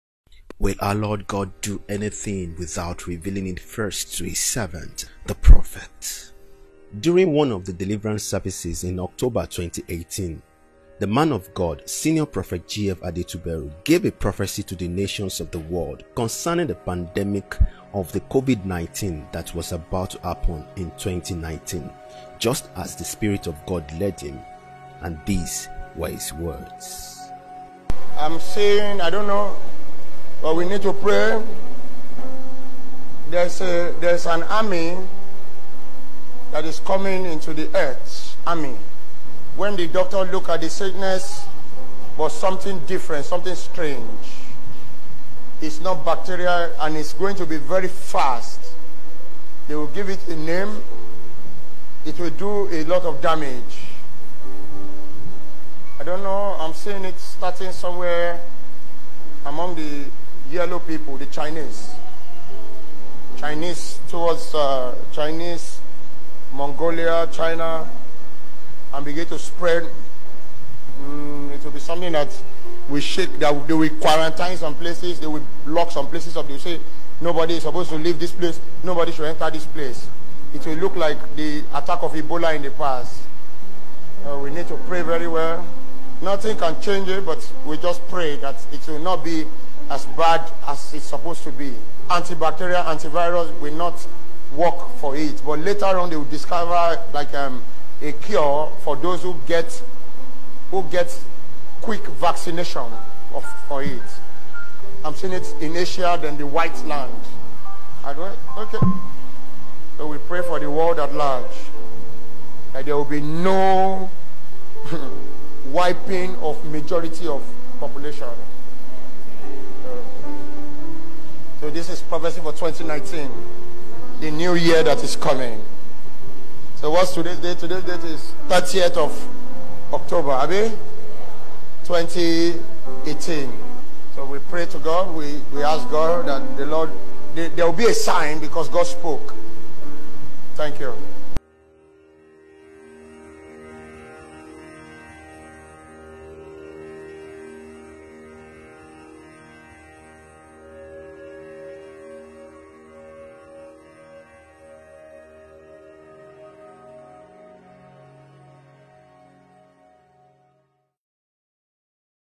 Pastor's Who Prediction As Far Back As 2018
This videos shows a Nigerian pastor, prophet Adetuberu predicting during one of his church services in October 2018.
What are your thoughts? Let's know in the comments section.